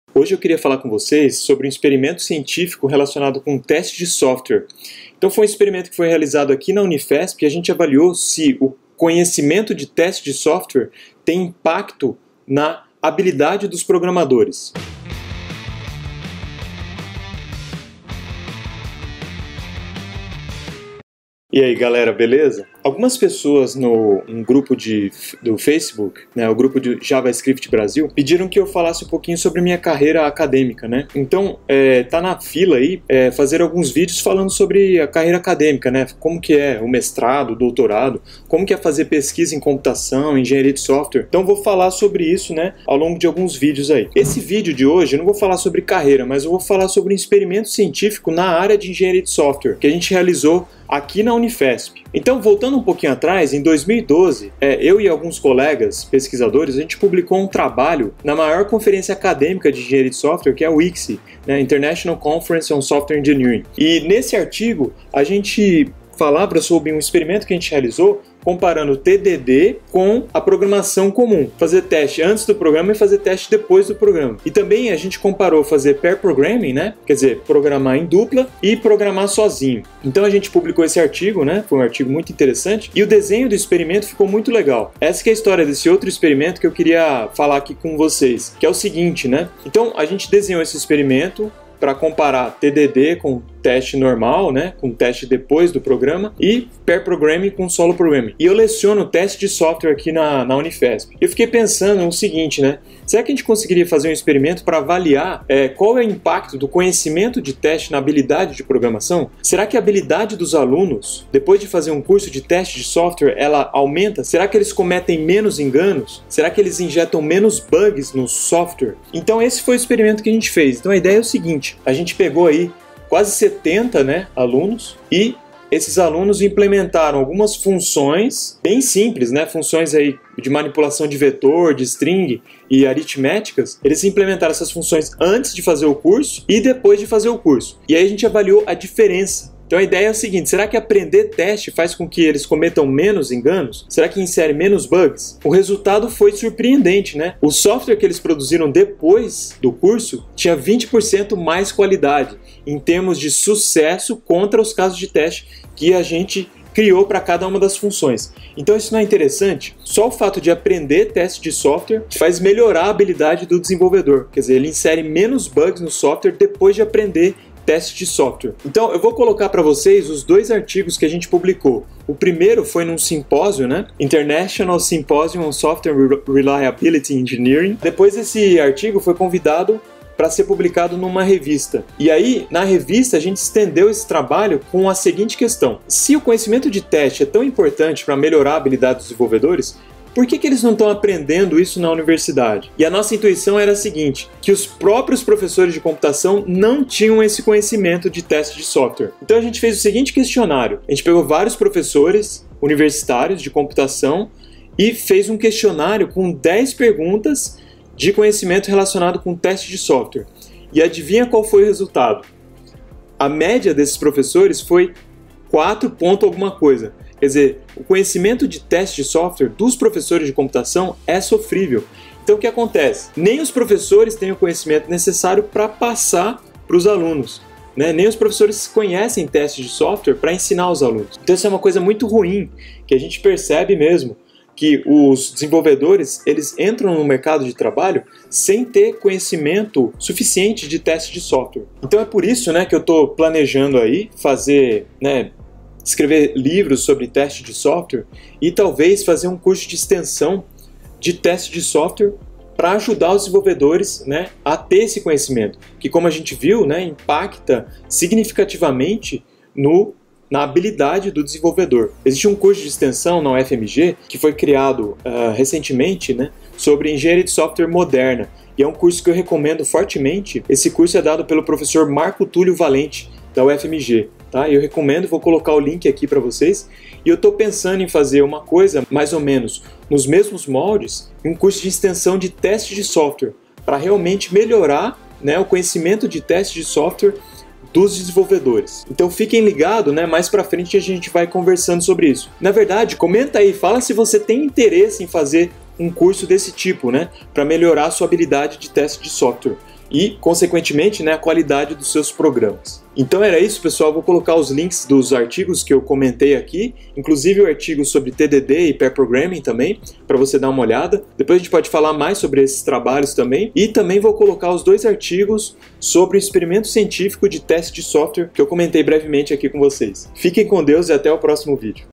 33 - Ciência do Software
Você sabe o que faz um pesquisador de engenharia de software? Nesse vídeo eu dou dois exemplos de trabalhos científicos que eu e meus colegas publicamos, com ênfase em um trabalho de teste de software. Nesse trabalho avaliamos o impacto do conhecimento de teste de software na habilidade do programador. Os resultados são bem interessantes, confira!

Quem tiver mais interesse nos trabalhos pode ler os artigos a partir de minha página pessoal (coloquei os pre-prints lá, que são versões quase completas antes da publicação, pois só se tem acesso aos completos a partir do sistema de uma universidade ou pagando). Os links estão ao lado dos artigos como "pre-print" na seção SELECTED PUBLICATIONS

O curso de extensão Engenharia de Software Moderna do Prof. Marco Tulio pode ser acessado O livro escrito por ele pode ser lido gratuitamente a partir da mesma página!

Referências:
Development of auxiliary functions: Should you be agile? An empirical assessment of pair programming and test-first programming

Can software testing education lead to more reliable code

The impact of Software Testing education on code reliability: An empirical assessment